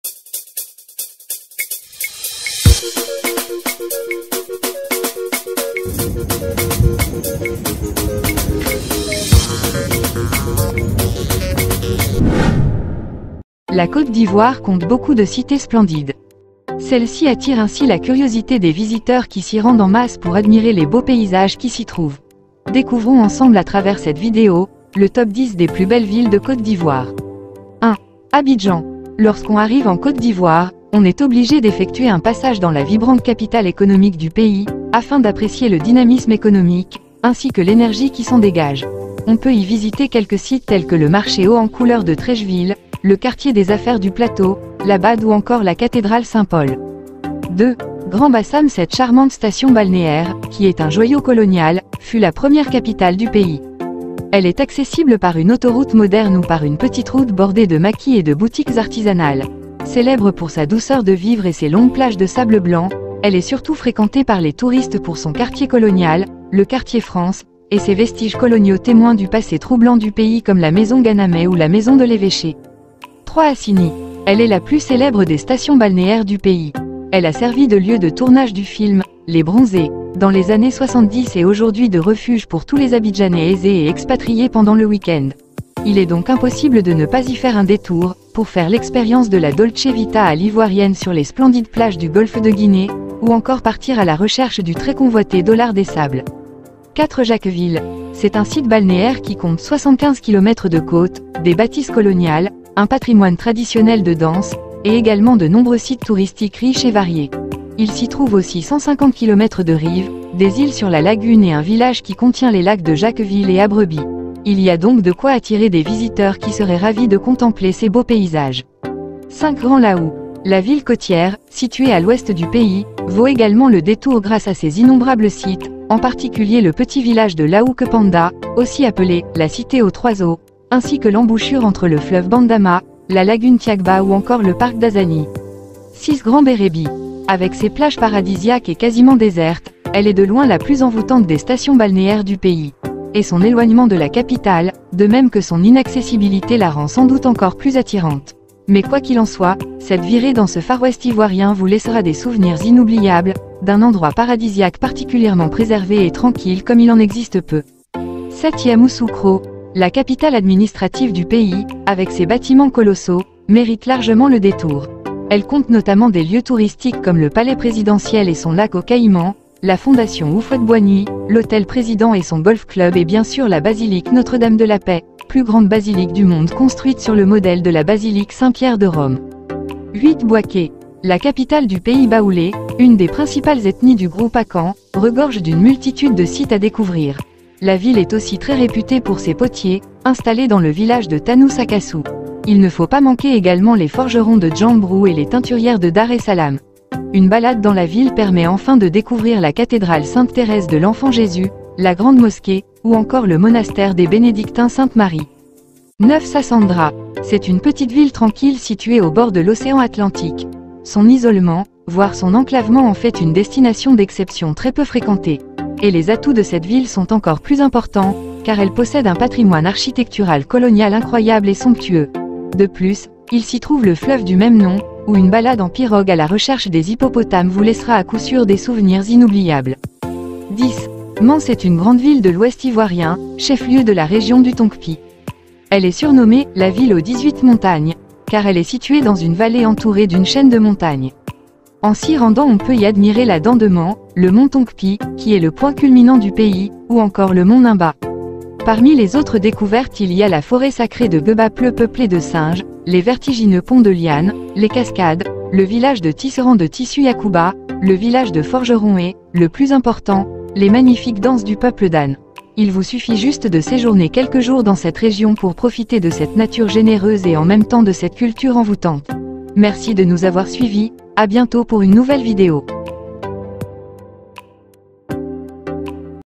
Les #10 belles villes# de la Côte d'Ivoire qui vous émerveilleront
Découvrez les joyaux cachés de la Côte d'Ivoire avec notre sélection des 10 plus belles villes du pays. Des plages de sable fin aux bâtiments historiques, en passant par la richesse culturelle et les paysages époustouflants, chaque ville a son charme unique à offrir aux voyageurs curieux. Explorez la diversité de ce magnifique pays d'Afrique de l'Ouest et laissez-vous séduire par la beauté et l'hospitalité de ces destinations inoubliables.